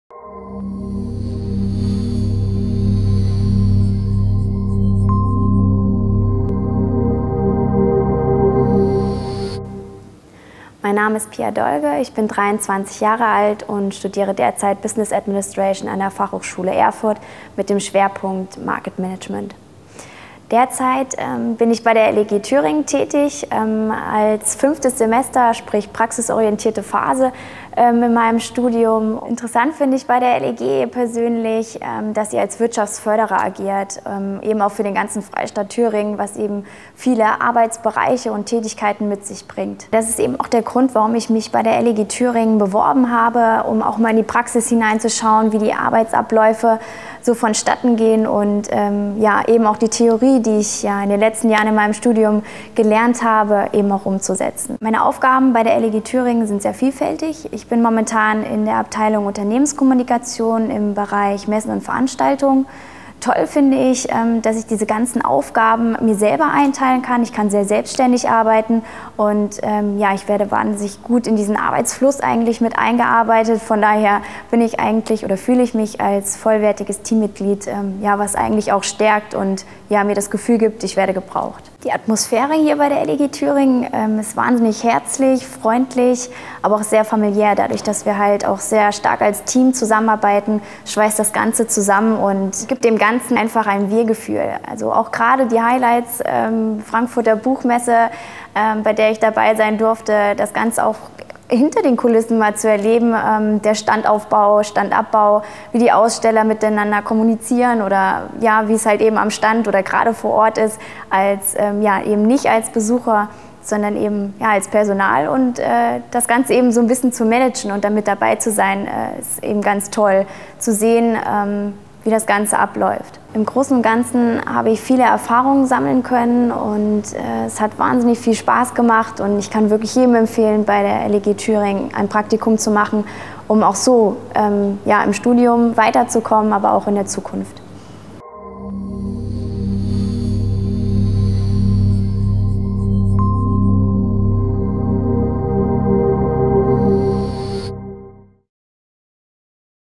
Praktikanten bei der LEG Thüringen: Pia Dolge
Praktikantin Pia Dolge berichtet über Ihre Erfahrungen in der Abteilung Unternehmenskommunikation bei der LEG Thüringen.